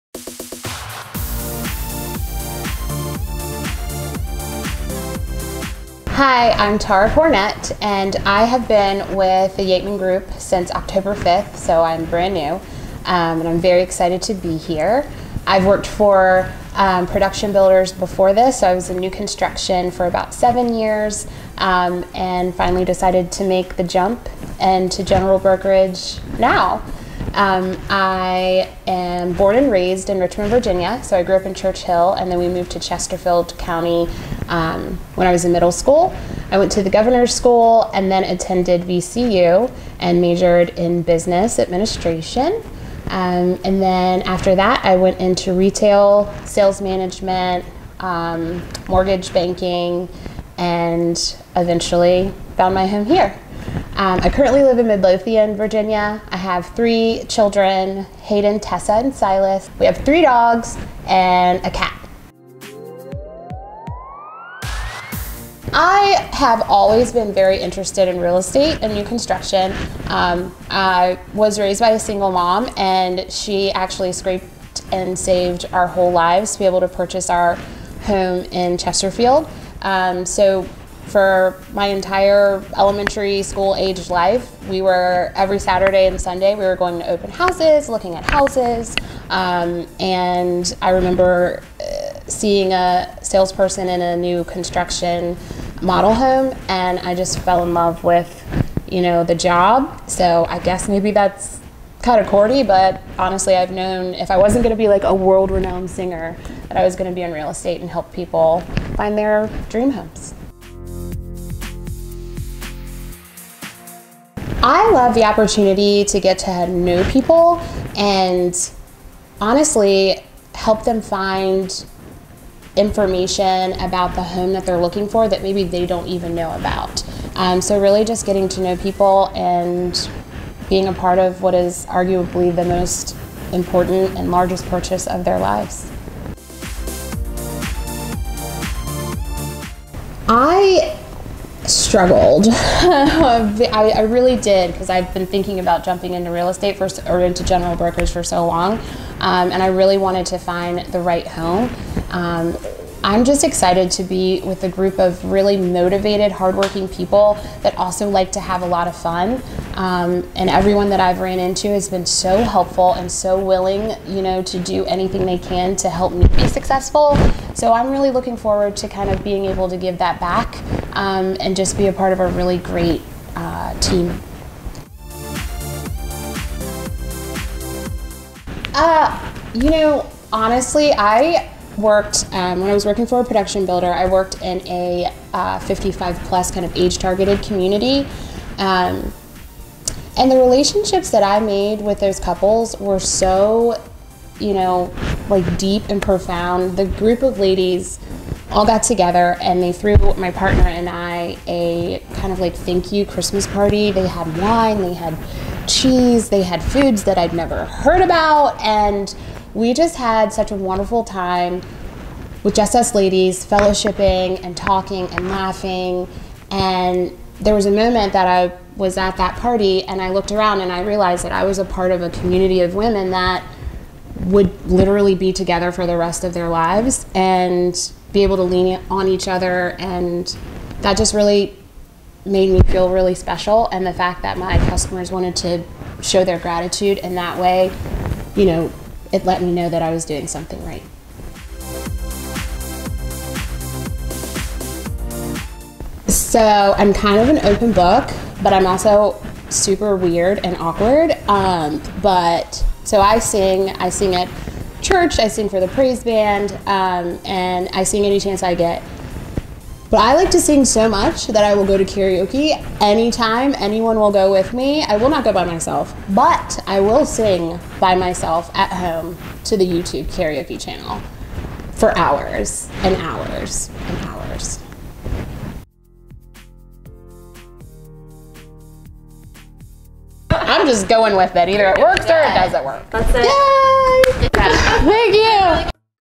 Meet Tara Cornett
Meet another one of our newest team members, Tara Cornett! Originally from Richmond, VA, Tara has worked as a Mortgage Loan Offices as well as a Senior Sales and Marketing Rep for a well-known local production builder. Find out a little bit more about her and her passion for helping people! Welcome Tara! 👋🏻